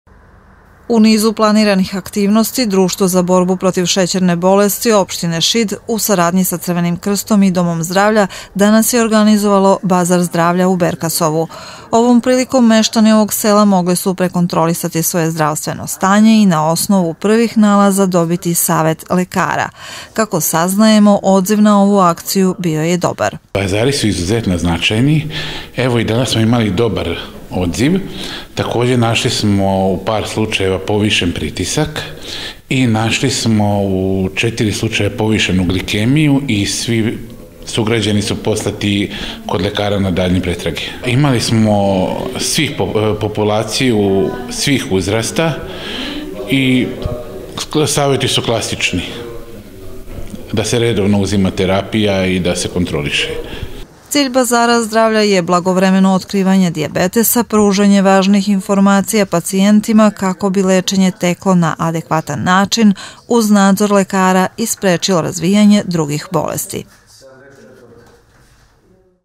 Bazar zdravlja u Berkasovu
U ambulanti  u Berksovu danas je održan  Bazar zdravlja. Meštani sela imali su  priliku  da provere svoje zdravstveno stanje a   Bazar su  zajednički organizovali   Crveni krst Šid, Društvo  za borbu protiv šećerne bolesti i Dom zdravlja Šid